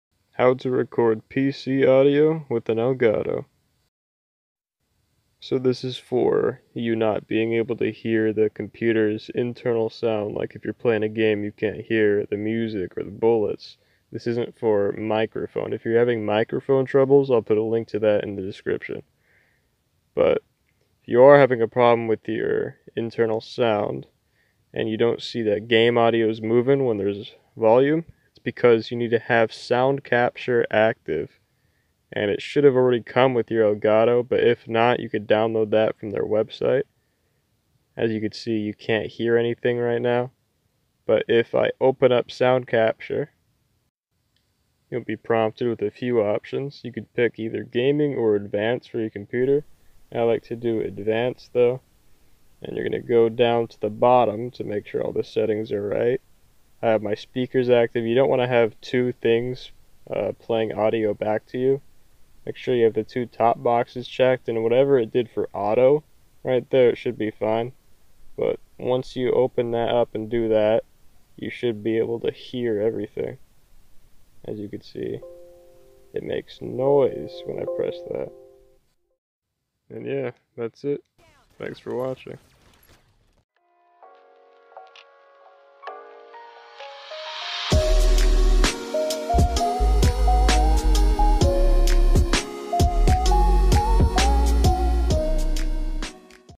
How To Record PC Audio With An Elgato
This is a speedy tutorial on how to record your internal computer audio with an Elgato.

Main Music: None

Line Username: APBsniper
Xbox: APBsniper